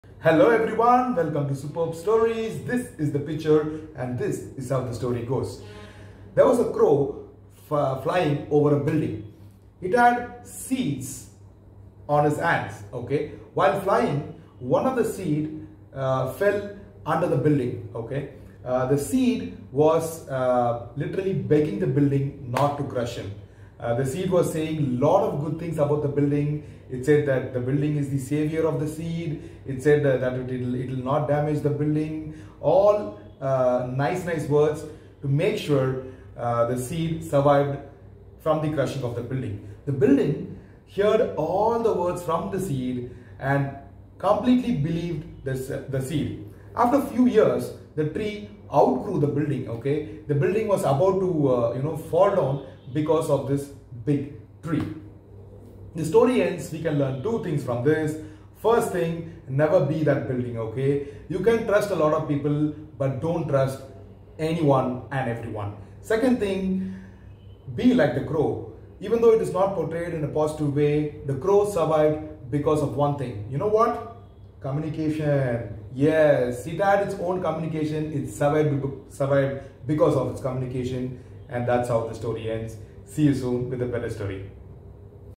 SuperbStory of Crow & the Building
A story of A Crow & a Building.
Importance of Believing People & Communication is the main Takeaway from this Story😍